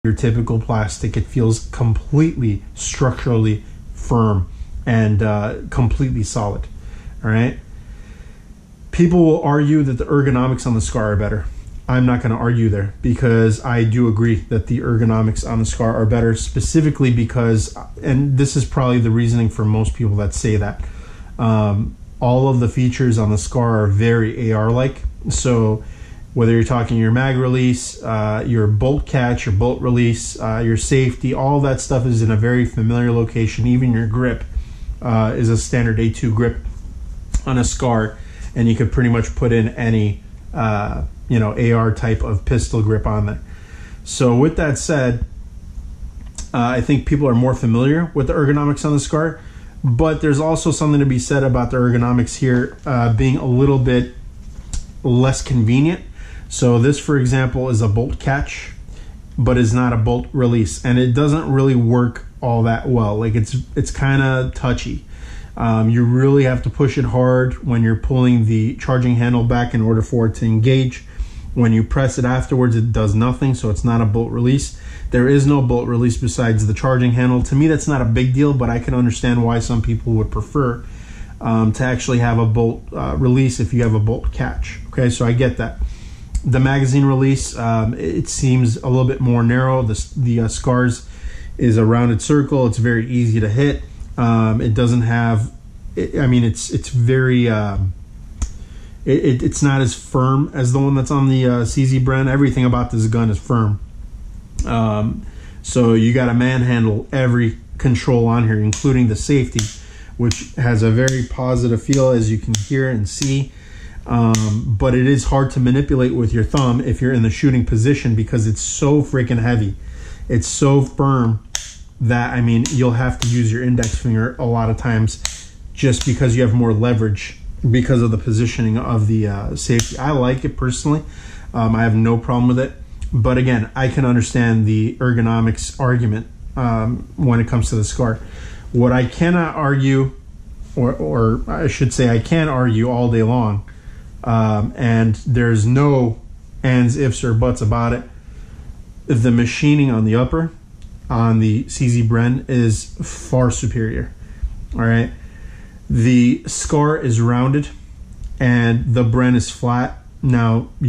PART 2: CZ SCORPION EVO AND CZ BREN 805 PISTOLS REVIEW
Forgive me for the 3 parts of 1 video.  I couldn't load all the content into one file.

Reviewing my new CZ Scorpion Evo S1 and CZ BREN 805 Pistols.  Diving deep into build quality, fit and finish, shootability, and comparing the BREN to the FN SCAR.